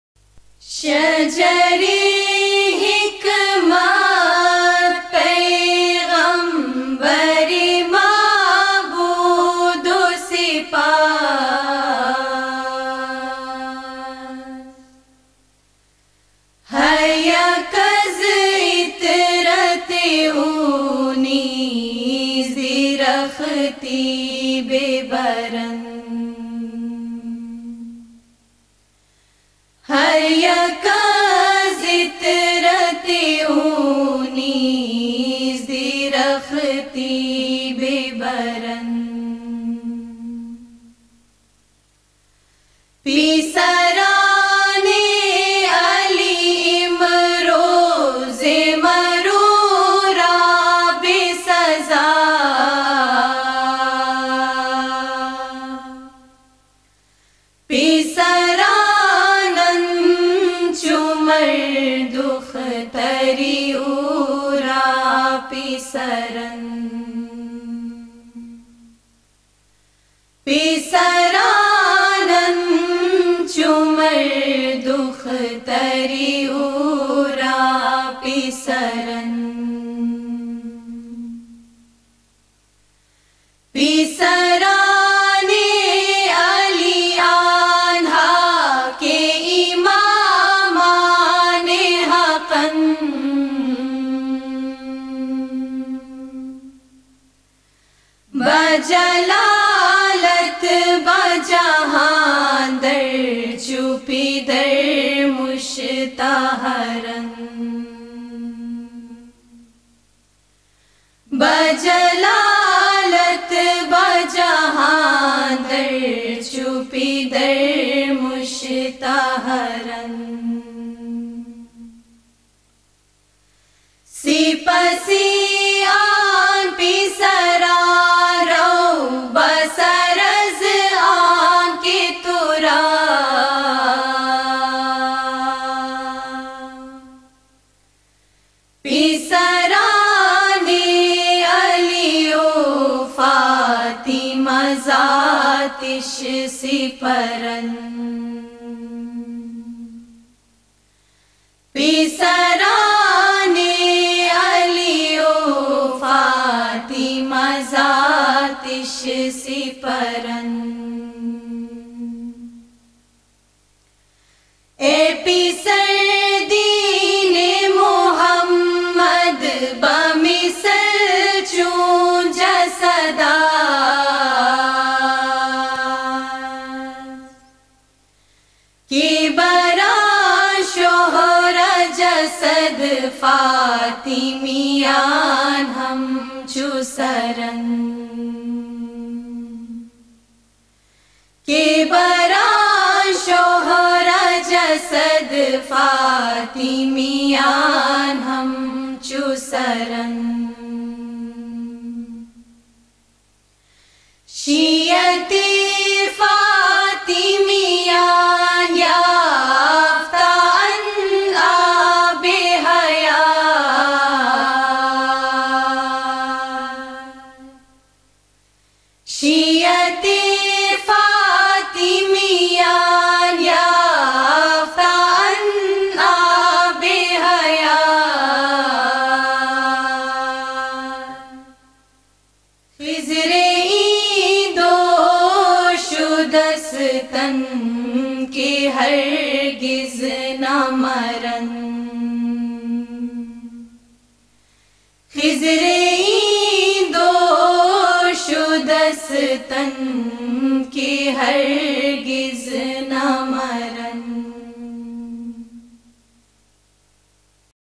Farsi Ginan | Ismaili
Ismaili Manqabat by Peer Nasir e Khishraw
During Didar Mubarak 2017. Aliabad Hunza.

Credits: Ismaili council for Pakistan